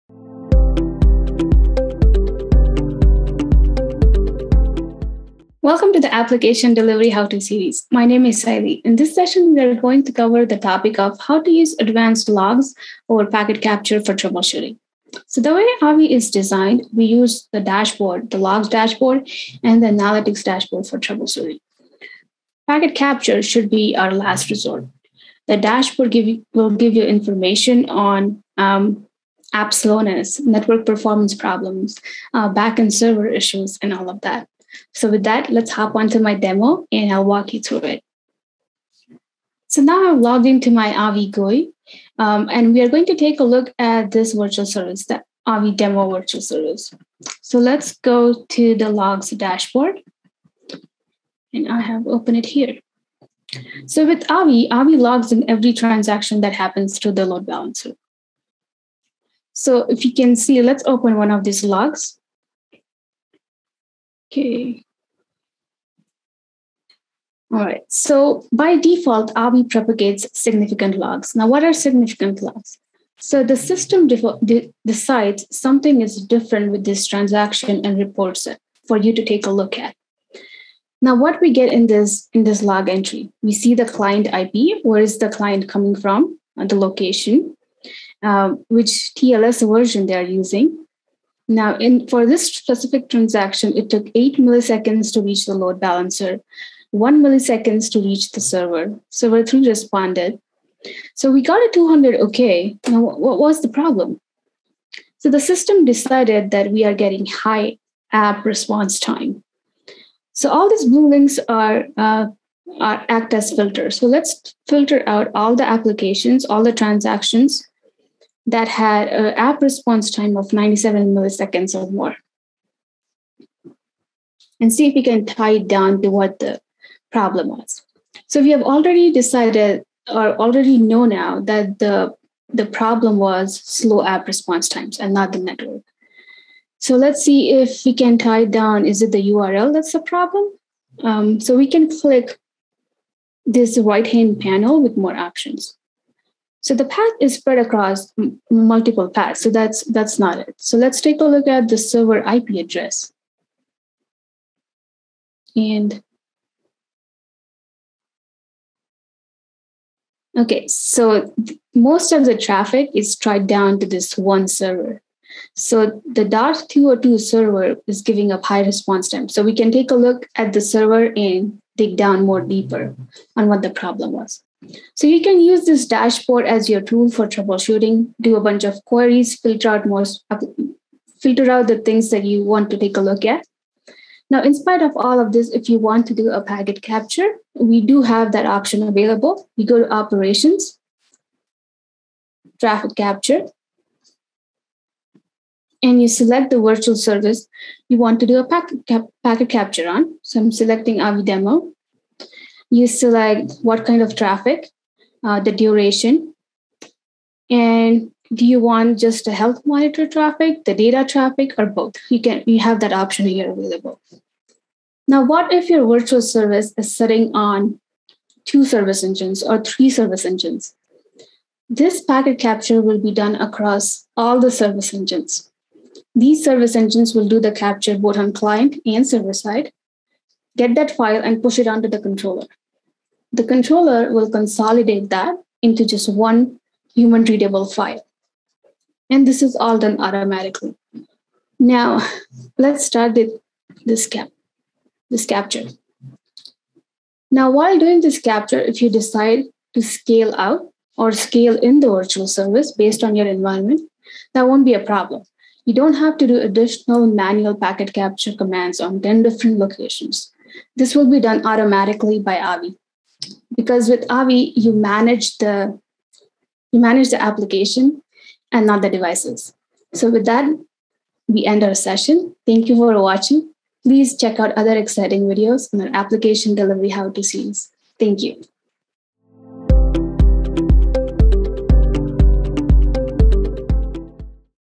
How to Use Advanced Logs and Analytics Over Packet Capture for Troubleshooting
Packet capture has been around for decades but it should really be your last resort. In this video, it shows how to use advanced logs and analytics to troubleshoot in an easier and more real-time fashion.